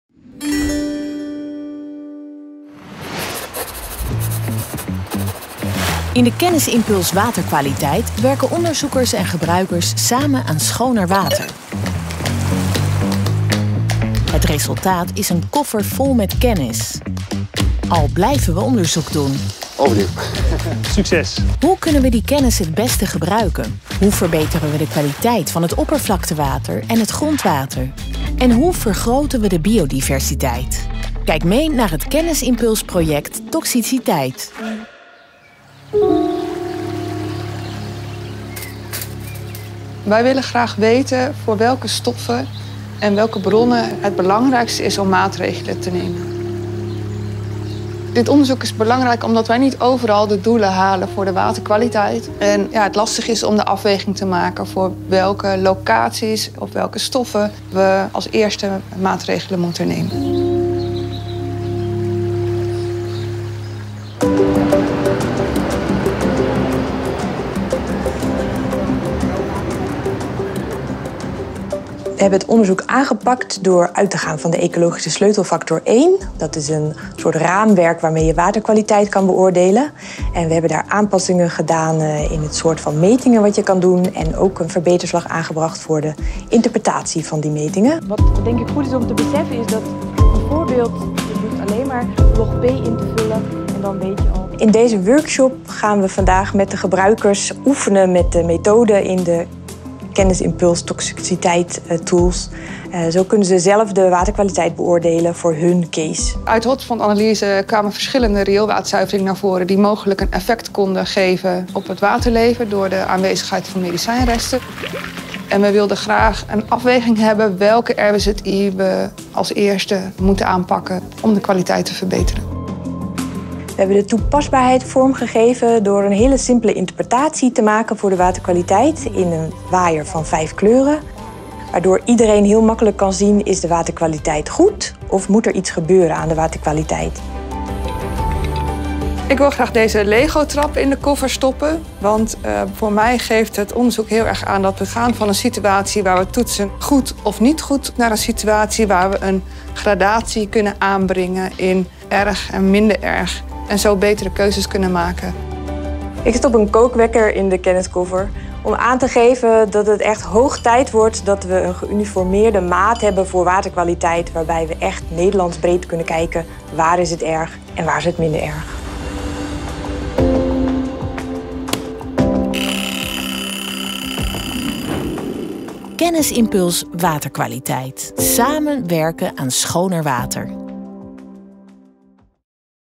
Waterkwaliteit | Kennisimpuls Waterkwaliteit, Toxiciteit | 2022
In de Kennisimpuls Waterkwaliteit hebben Rijk, provincies, waterschappen, drinkwaterbedrijven en kennisinstituten gezamenlijk gewerkt aan het vergroten van het inzicht in de kwaliteit van het grond- en oppervlaktewater en in de factoren die deze kwaliteit beïnvloeden. Daarmee kunnen waterbeheerders de juiste maatregelen nemen om de waterkwaliteit te verbeteren en de biodiversiteit te vergroten.
In het programma zijn bestaande kennis en inzichten samengebracht, en waar nodig is nieuwe kennis ontwikkeld. Deze kennis is ook (beter) toepasbaar gemaakt voor de praktijk van het waterbeheer, in de vorm van praktisch toepasbare tools en instrumenten. Deze pagina geeft toegang tot alle resultaten van de Kennisimpuls.

Meer weten over de Kennisimpuls Waterkwaliteit? Ga naar
Meer weten over dit project? Ga naar